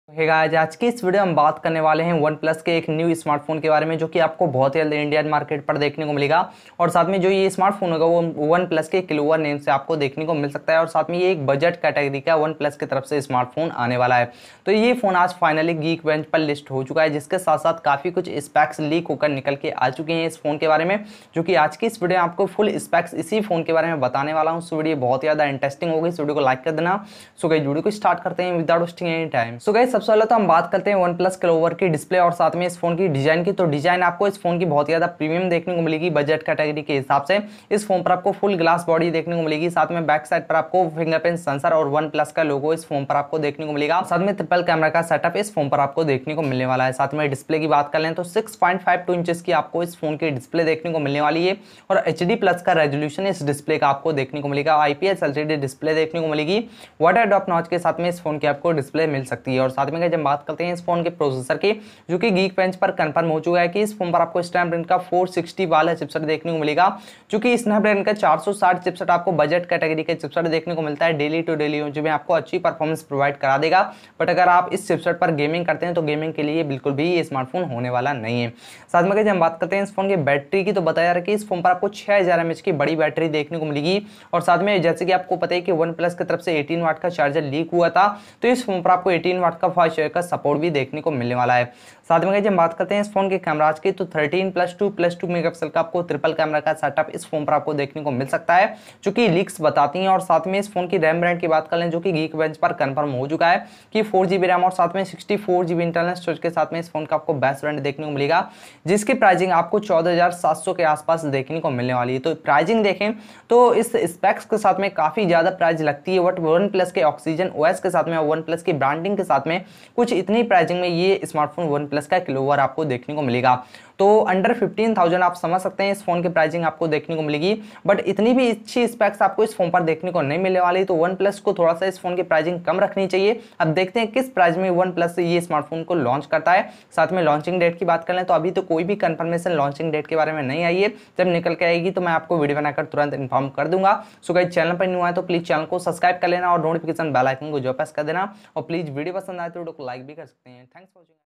OnePlus Clover - Launch date, Price, Specification | OnePlus Nord lite
So guys ajj ki is vedio me apko btaunga OnePlus Clover ke bare me OnePlus Clover  price, OnePlus Clover launch date,OnePlus Clover Specification and all information in this vedio...